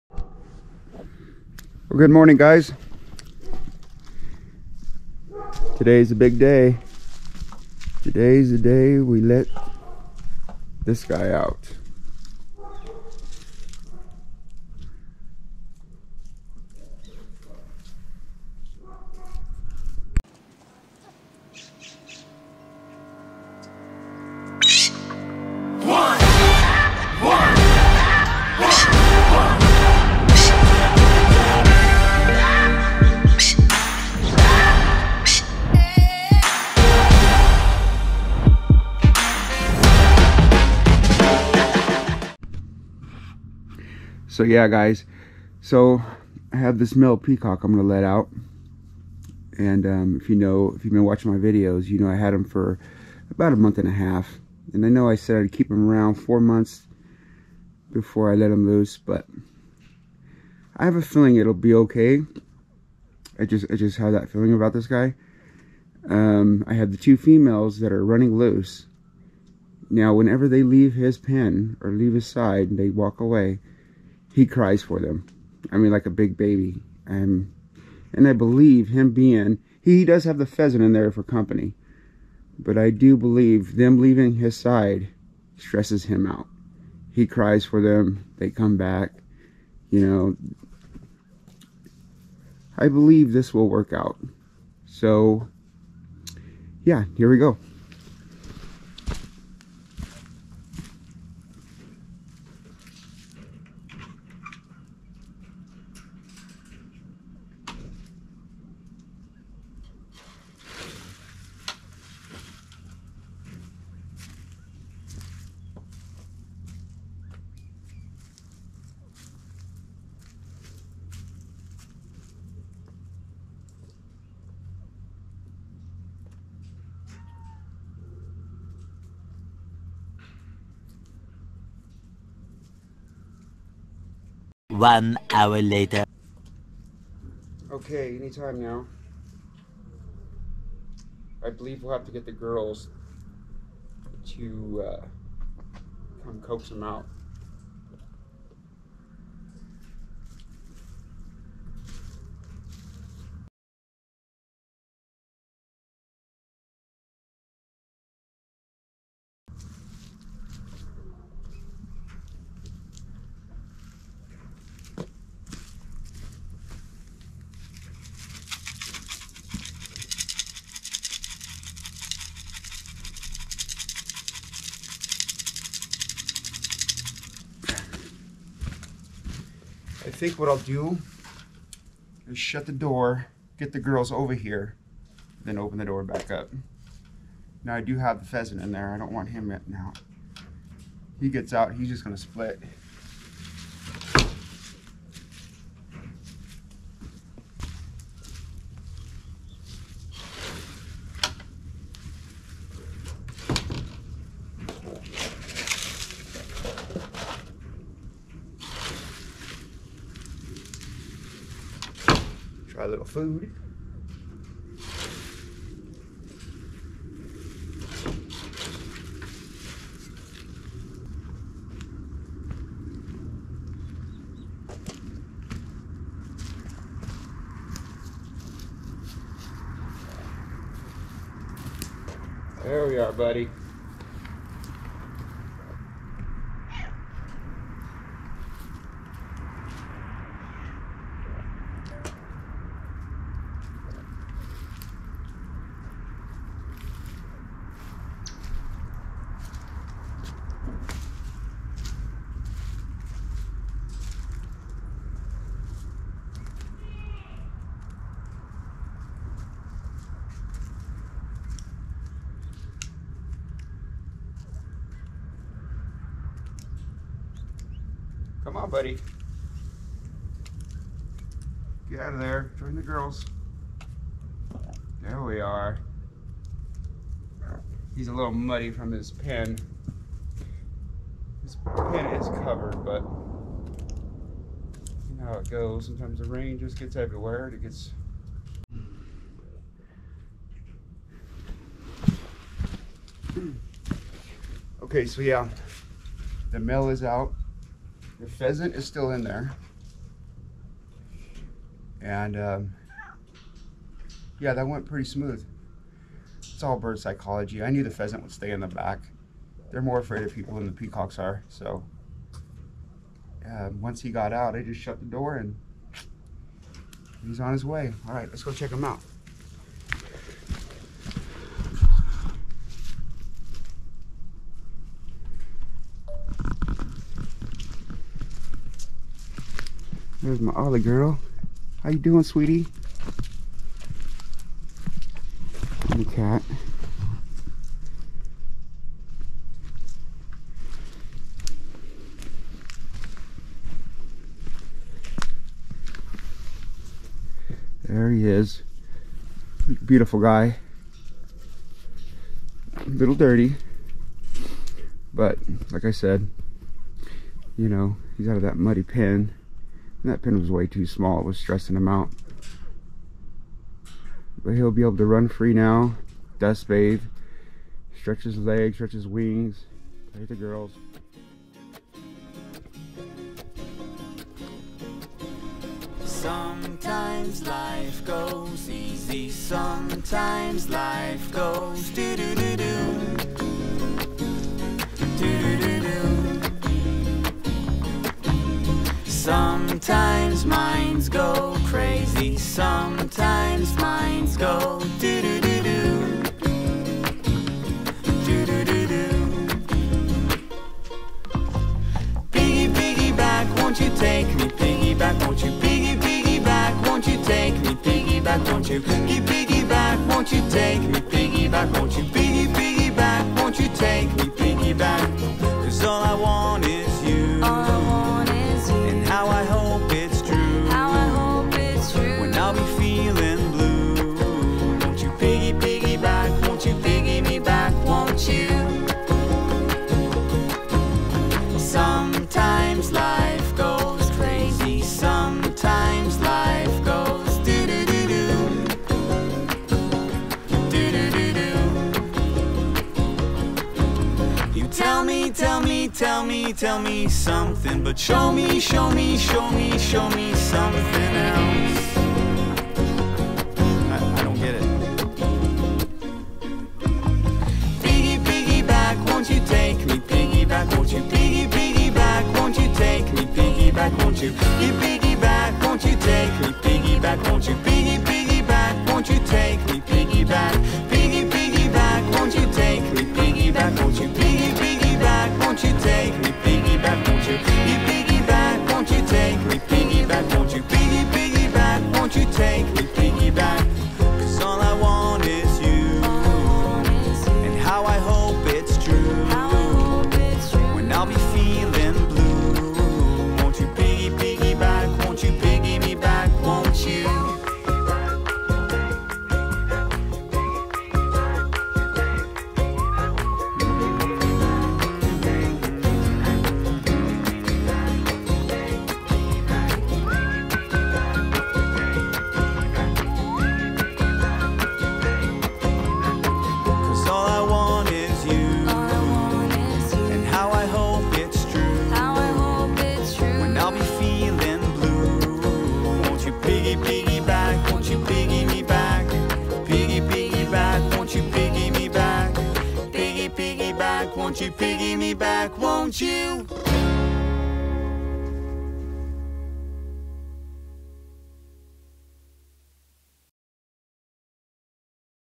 The White Peacock Is Free!
I know I said I would wait a few months before releasing the new peacock, and generally I would. But he needed to get out of that pen and with the peahens. He had a pheasant in with him to keep him company but he cried for the girls whenever they left his sight. Once he was let out, he just hung out with them like he's been with them for years. So far so good with his new found freedom. Enjoy!